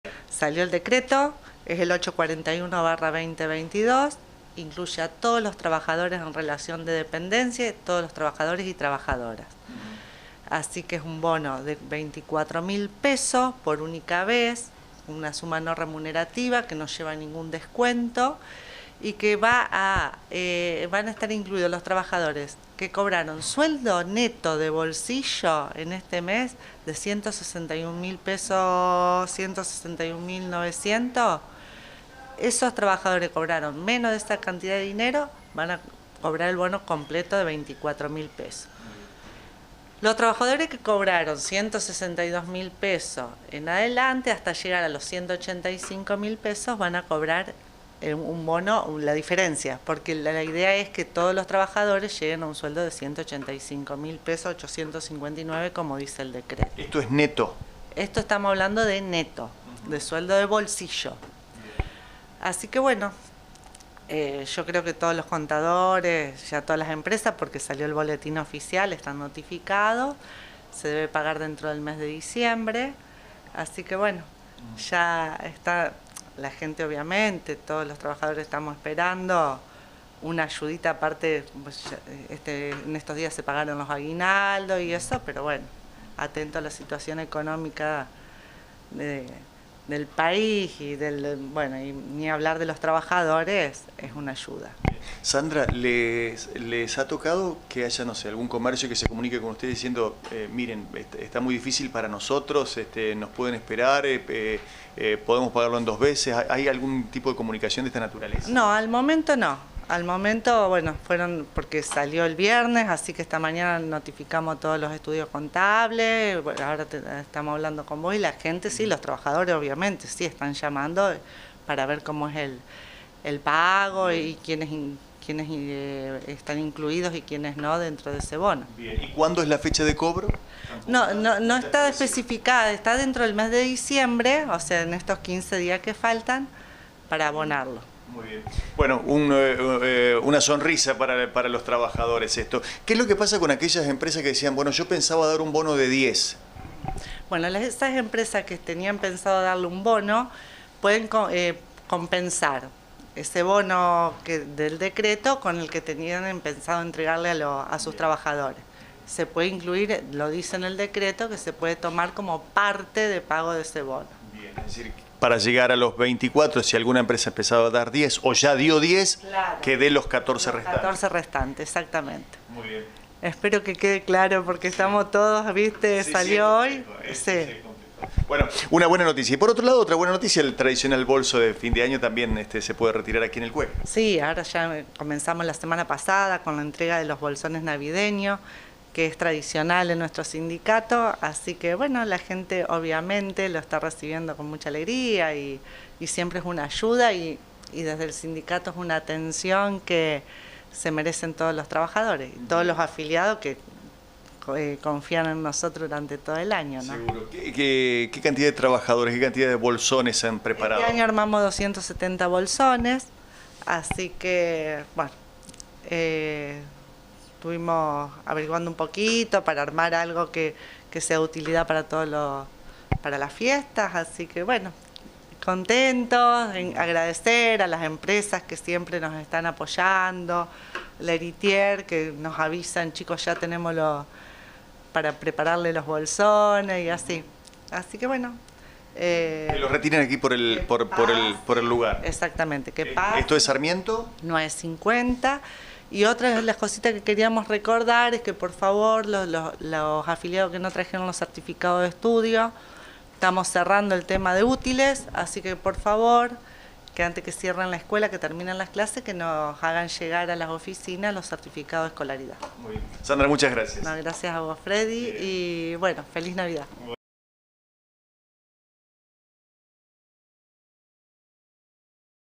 CUEC - ENTREGA DE BOLSÓN NAVIDEÑO PARA AFILIADOS Y BONO DE FIN DE AÑO PARA LOS EMPLEADOS.
SANDRA IMSAND -